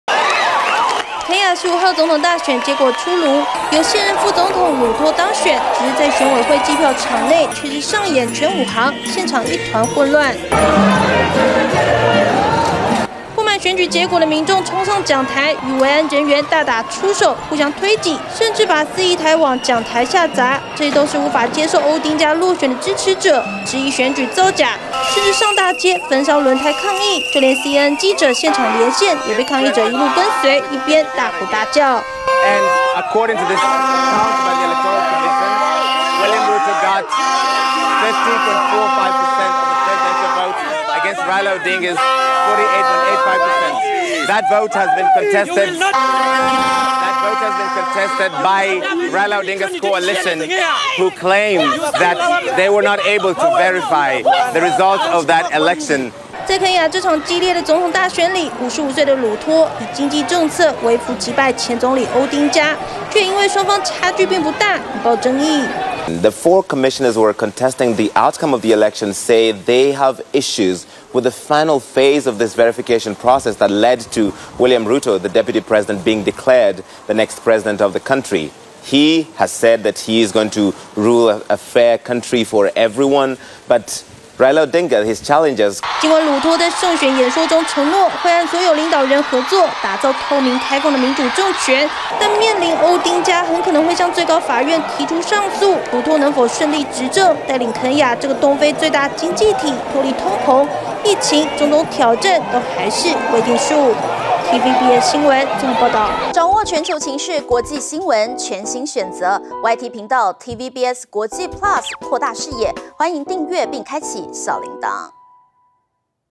質疑總統大選舞弊 肯亞反對派支持者怒砸計票中心｜TVBS新聞
東非國家肯亞，在當地時間15日總統大選結果出爐，由現任副總統魯托以微幅差距擊敗反對黨候選人歐丁嘉，卻引來歐丁嘉支持者不滿，認為選舉舞弊，不僅大鬧選委會計票中心，與魯托支持者打成一團，街頭也出現焚燒輪胎的抗議場面。